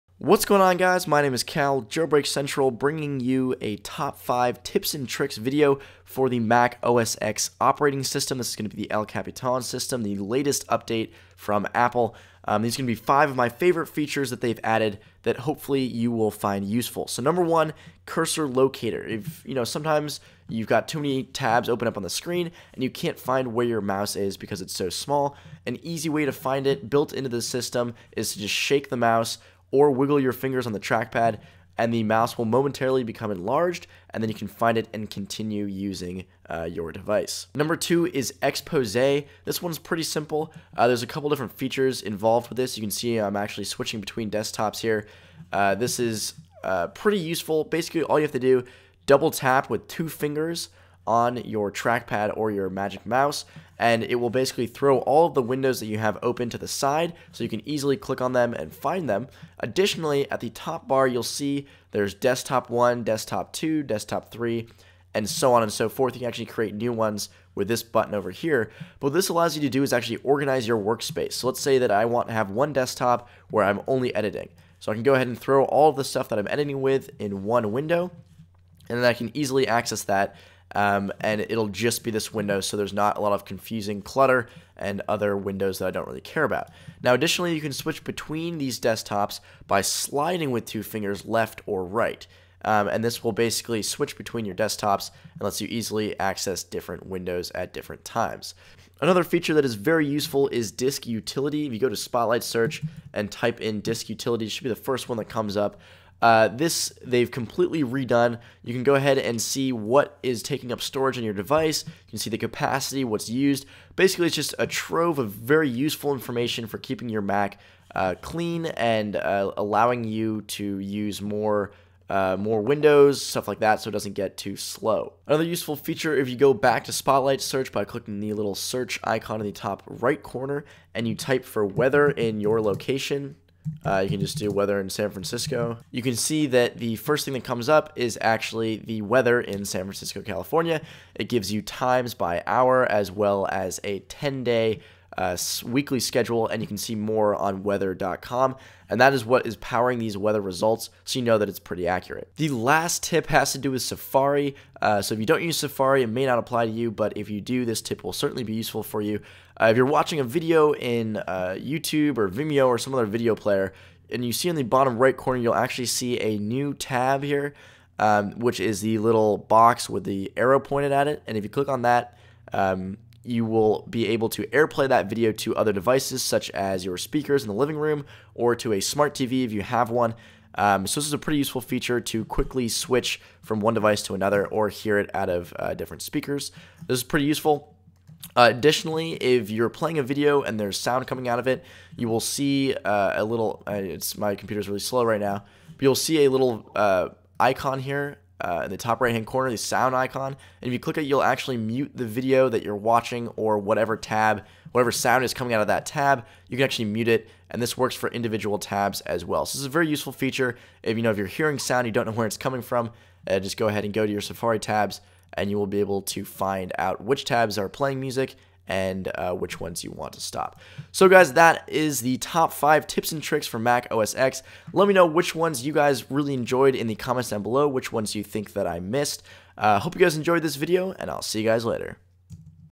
Mac OS X El Capitan: Top 5 Tips and Tricks + Hidden Features
My top 5 hidden features of mac os x el capitan. Let me know which you liked the most in the comments!